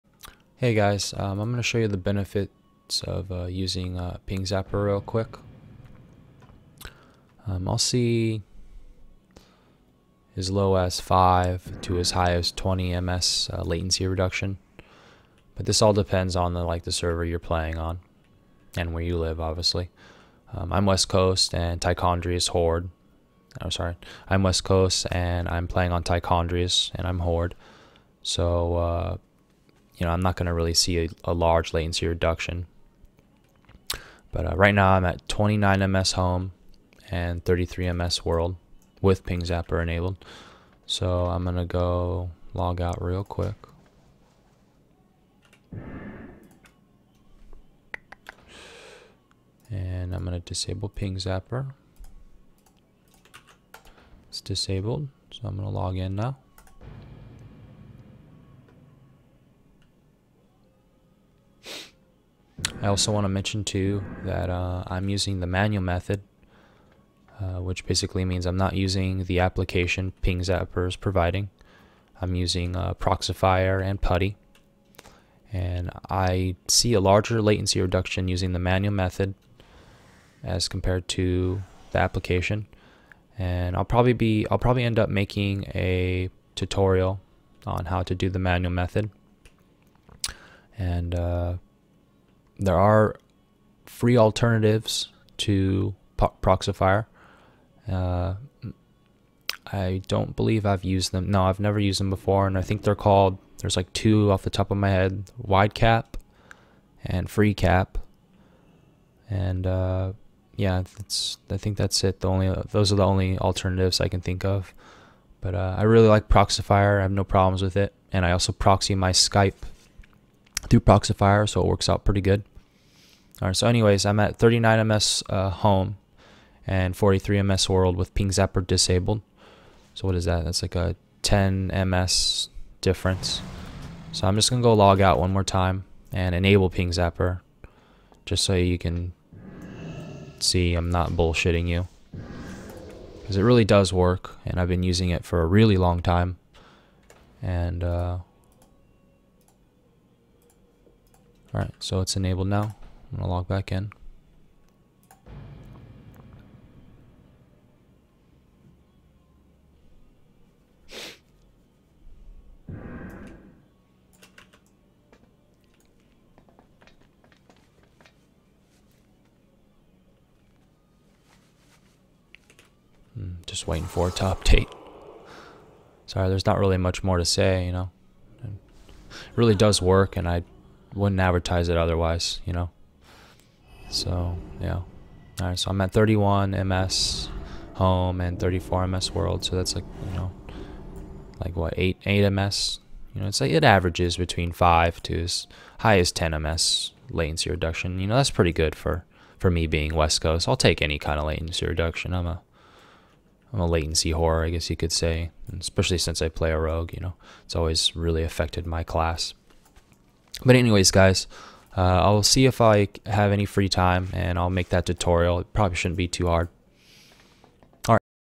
The benefits of Pingzapper
Showing the latency difference between Pingzapper enabled and disabled using the manual method.